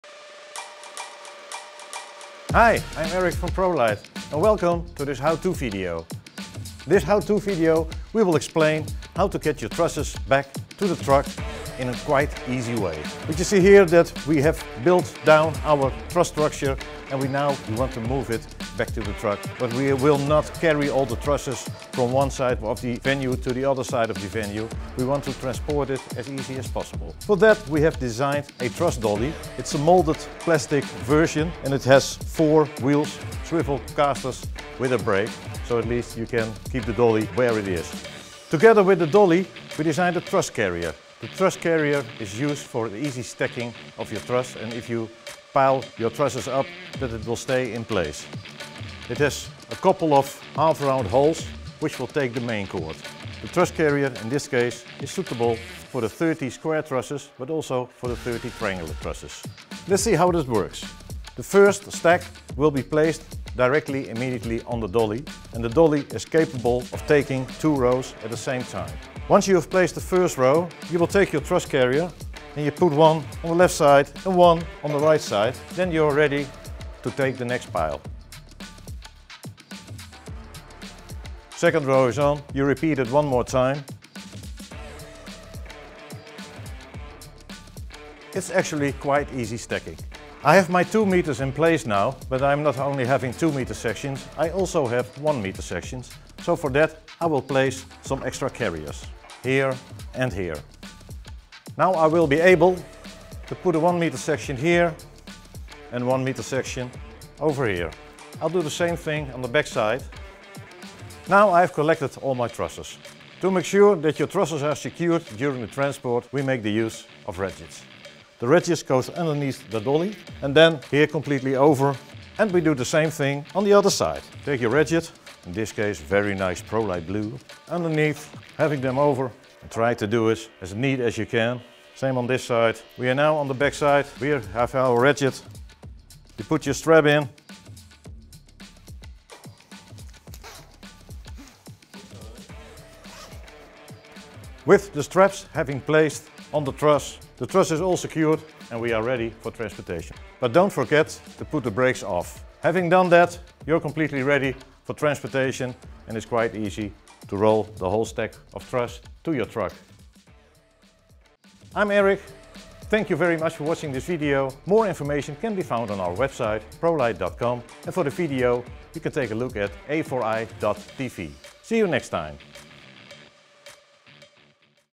Prolyte HOW-TO-VIDEOS: Episode 5: How to carry a truss?
Learn the proper techniques for safely and easily carrying and handling truss. In this tutorial, we'll guide you through best practices to ensure safety and efficiency when working with carrying truss systems.